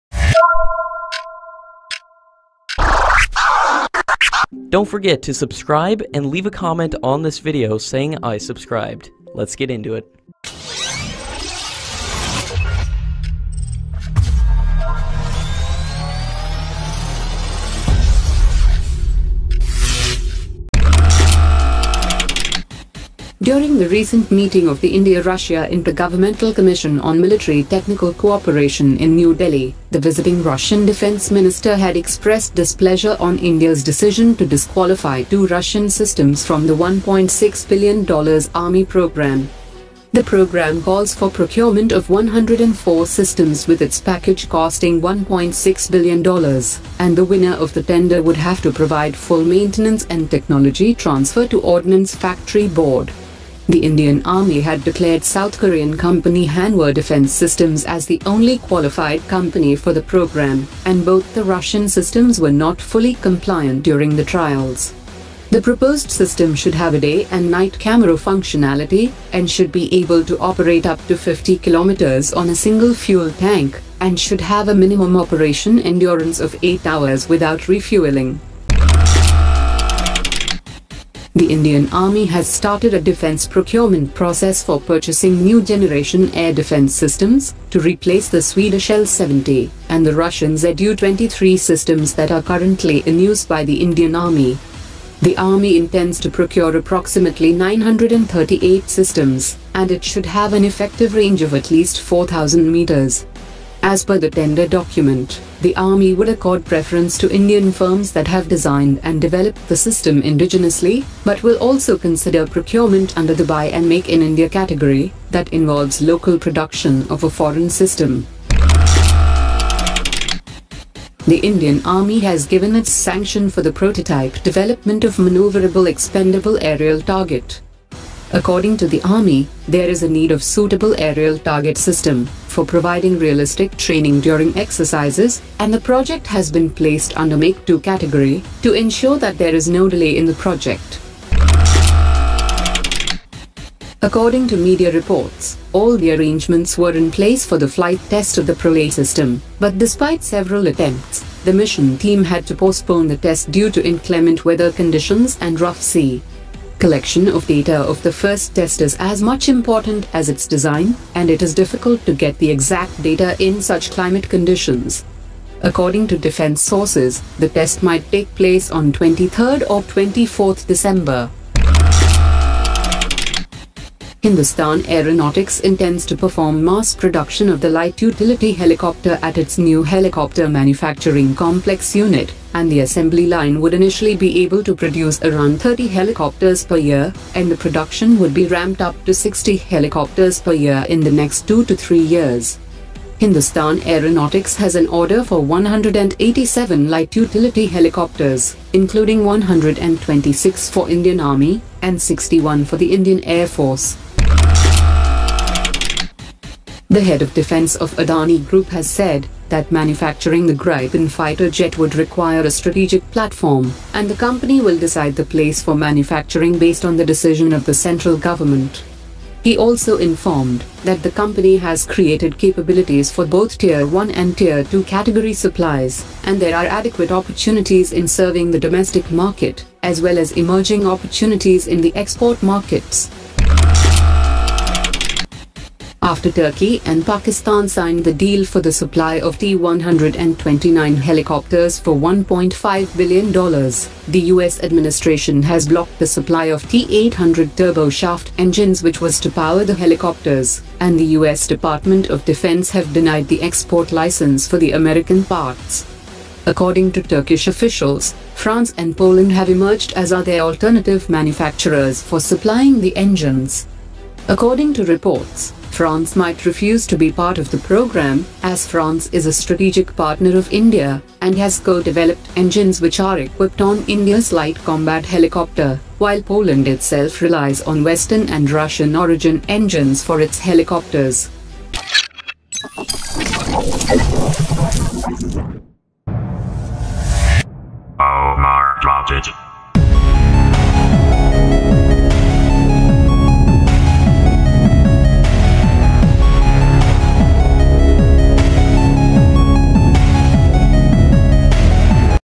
Indian Defence Updates : Army Selects Hybrid BIHO,Pralay SRBM Trials,RFI for 938 Air Defence Systems
1. Russia wants back in on Indian Army’s competition for $1.6 Bn Air defence system program with Technology Transfer.
2. Indian Army issues global RFI for  $5bn worth 938 Air defence system deal.
3. Indian army to get indigenous Maneuverable Expendable Aerial UAV.
4. HAL to start mass production of Light Utility helicopters at new facility. HAL LUH
5. Adani Defence forging global ties : ready with tier 1 tier 2 suppliers for Gripen in Indian MMRCA .
6. Maiden trial of Pralay SRBM postponed due to weather.
7. Turkey needs to find a new engine for Pakistan’s T129 order